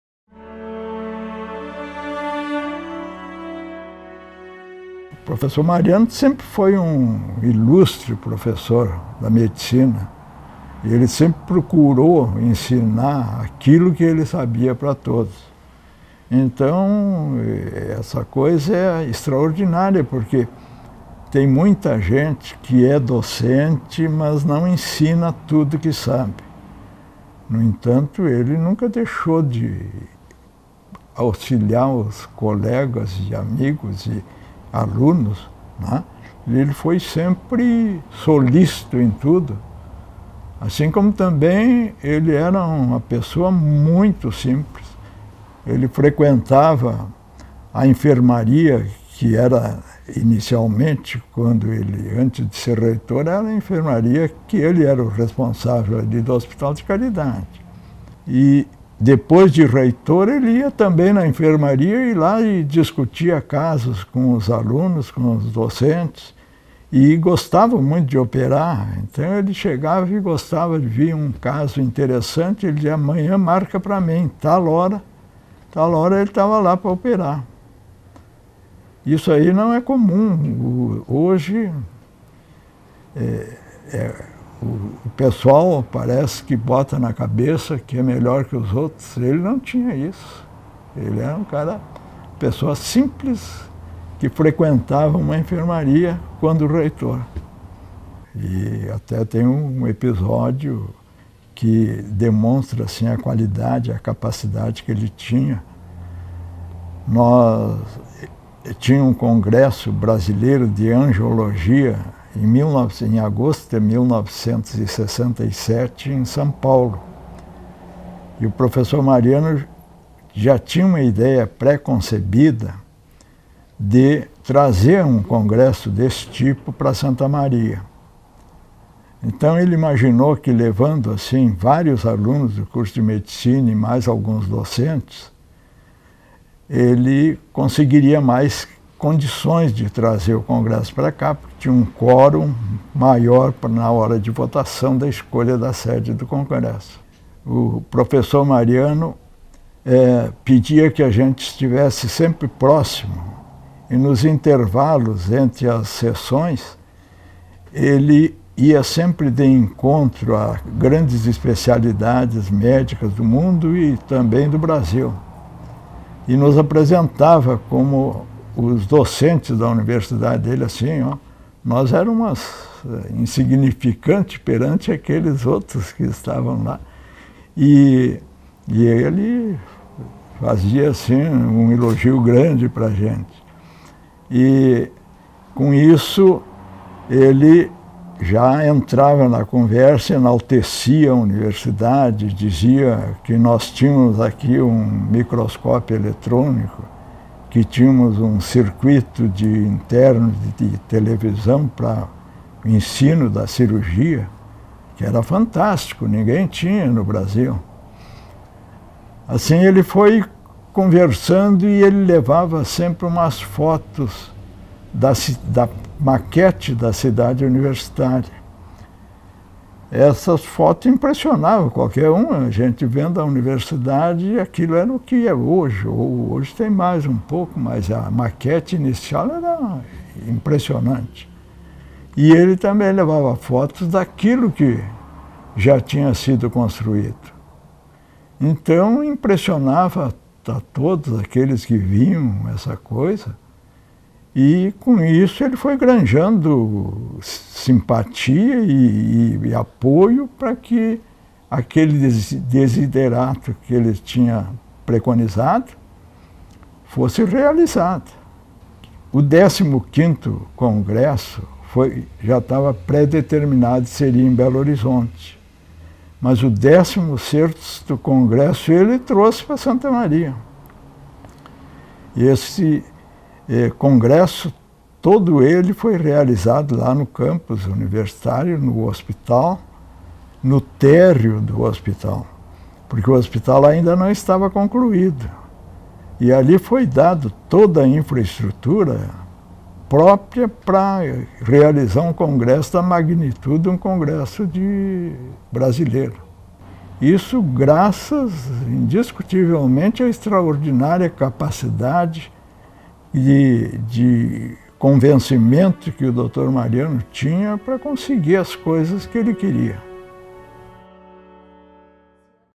José Mariano da Rocha Filho: 100 anos - Depoimento Valter Noal
Em fevereiro de 2016, encerram-se as comemorações do centenário de José Mariano da Rocha Filho, o fundador da Universidade Federal de Santa Maria.
Para fechar as homenagens a Mariano da Rocha, a TV Campus apresenta alguns depoimentos que relatam histórias e episódios relacionados a ele.